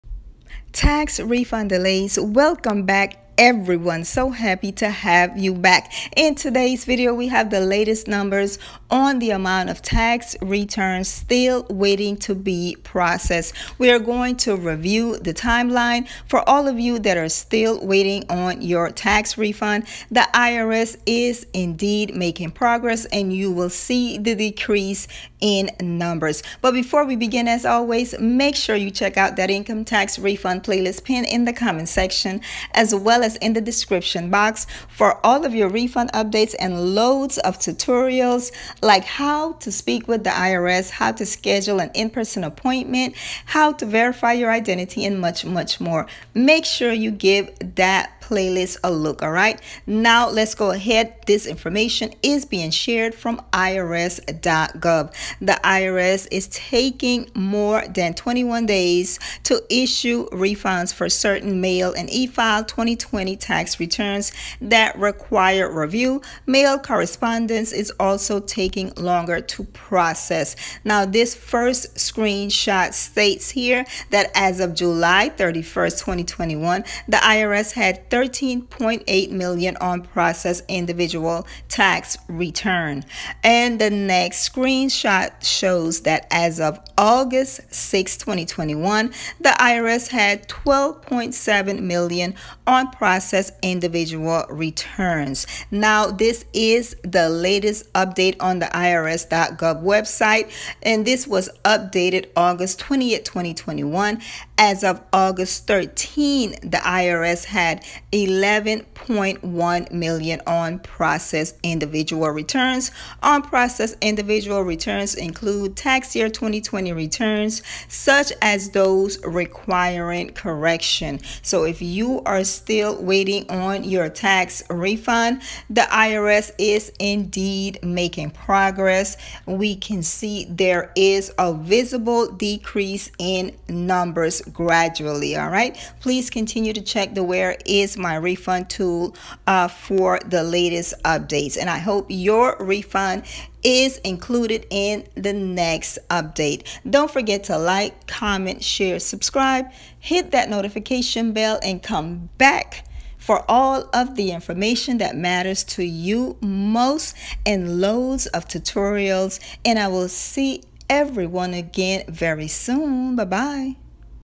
Tax refund update, latest numbers on tax refunds still waiting to be processed
Latest on Tax Refunds Still  Waiting To Be Processed

Here is the latest update as of 8/20/2021 from irs.gov on the tax refund delay
Number of tax refund still waiting to be processed

Where is my refund, and why is my refund still processing update.
The IRS is making progress approving tax refunds, but there are still
millions waiting to be processed especially manual tax returns..
Here are some tax refund approval dates shared in the comments
to give you an idea when your refund maybe approved.
Tax refund 2021. IRS tax refund 2021. Where's my refund 2021. Where is my refund

Sharing not an irs employee, can be found on irs.gov type in search amended return